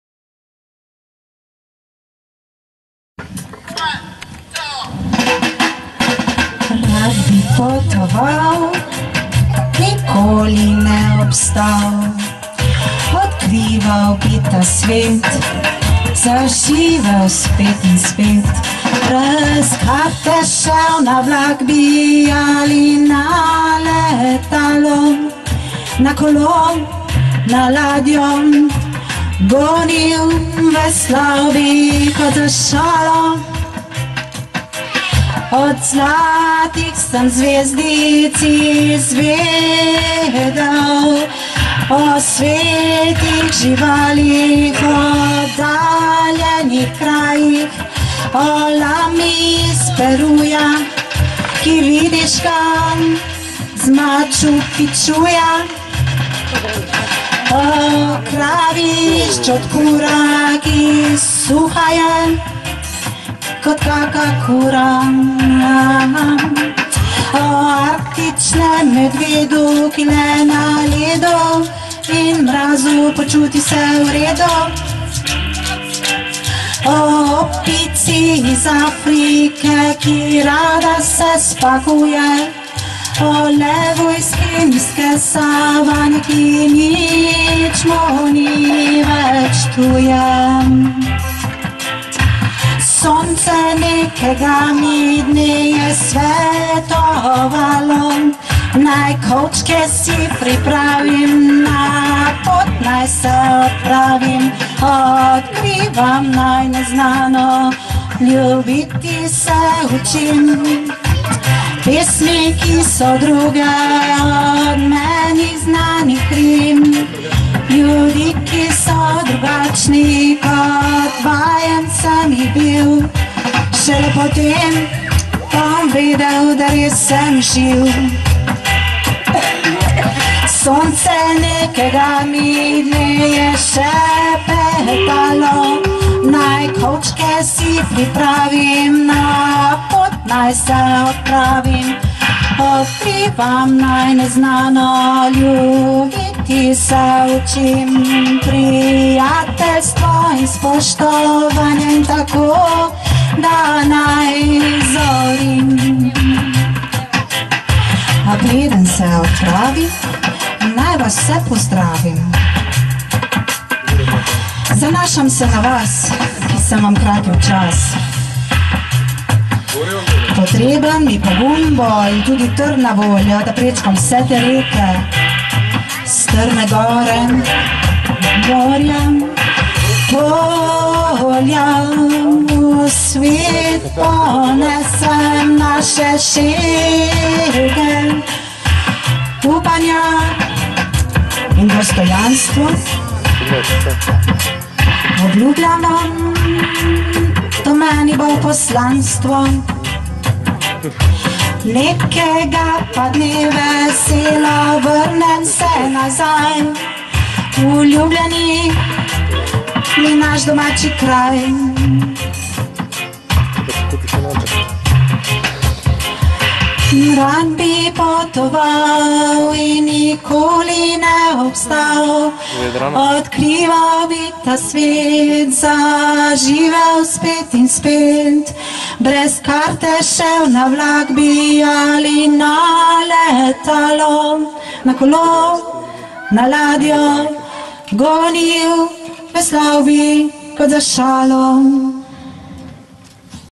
Čez drn in strn - 3. del
Performans Estelle Žutić in Gillesa Duviviera na rifniškem posestvu